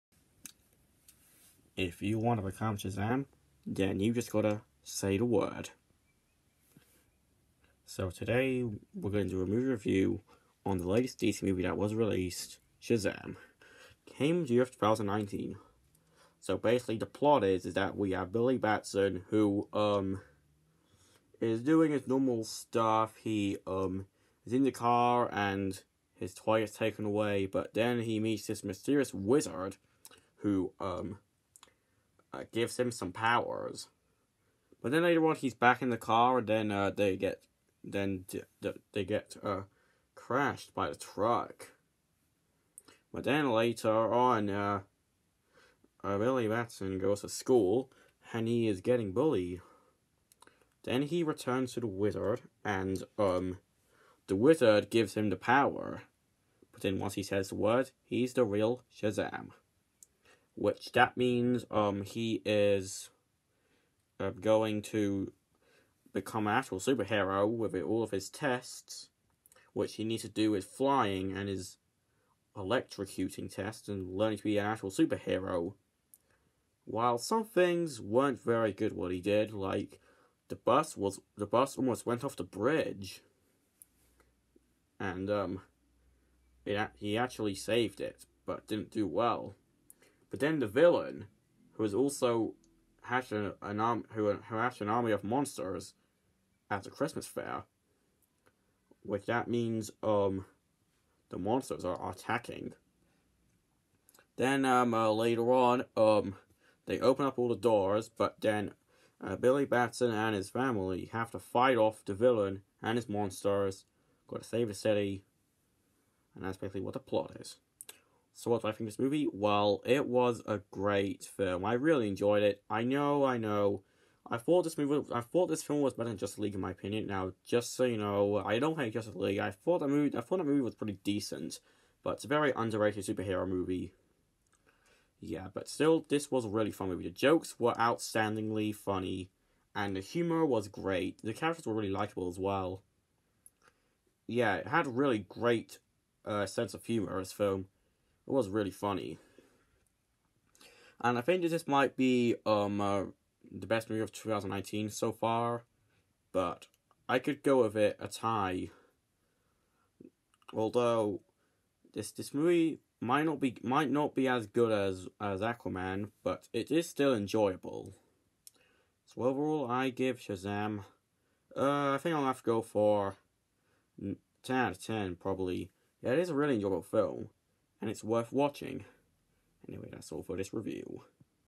Shazam (2019) Movie Review
I got back from seeing this film and I did a review on it. The only problem is that it’s not as good as Aquaman though. But still enjoyable.